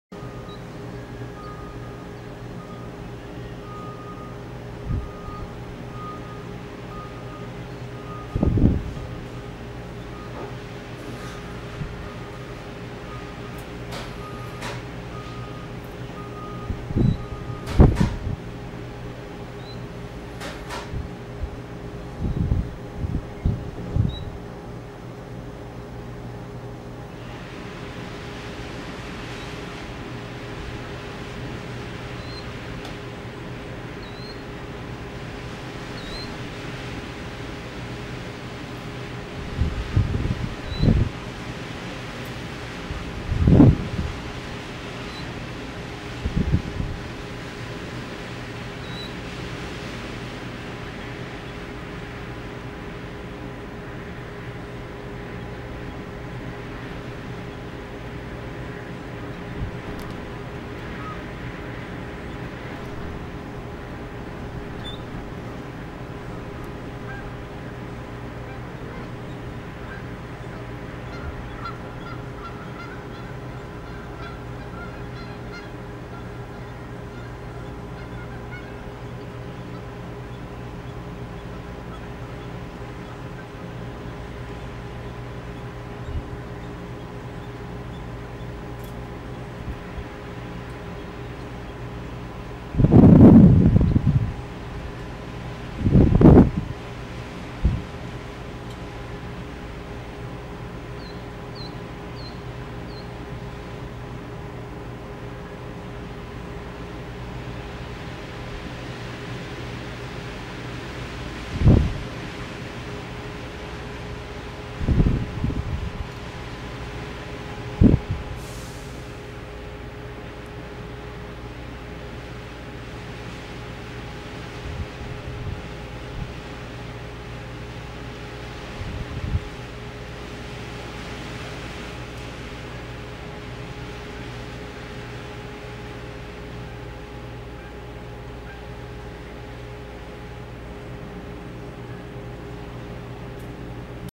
Chicks
I've been watching a Killdeer nest outside my office window on the roof for almost a month now. Yesterday 3 chicks hatched. It's been entertaining watching the mother try to keep them all in the nest...